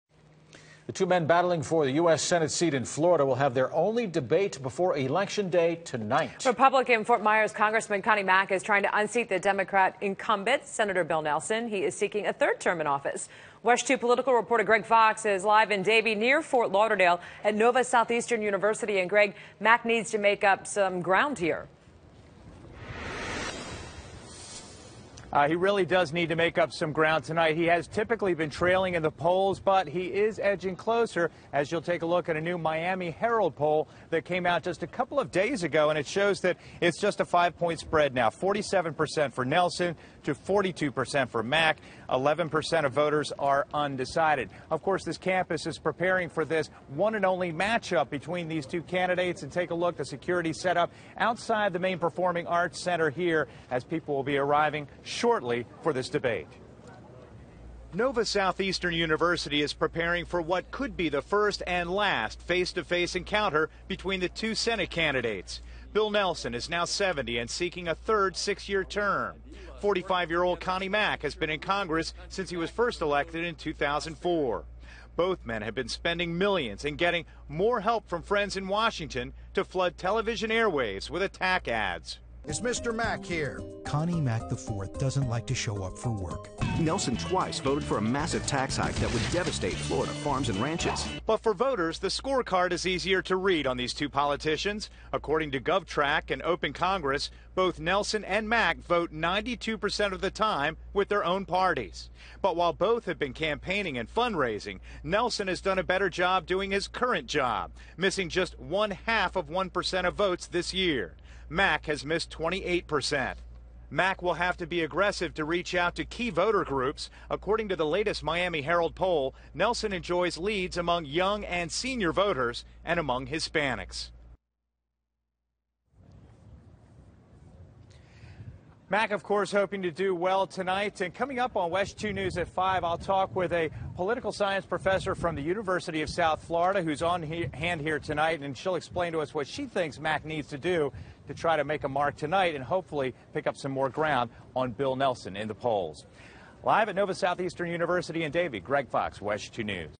Nelson, Mack to participate in first and only debate
Florida Sen. Bill Nelson and Rep. Connie Mack will debate over who should occupy Nelson's U.S. Senate seat next year.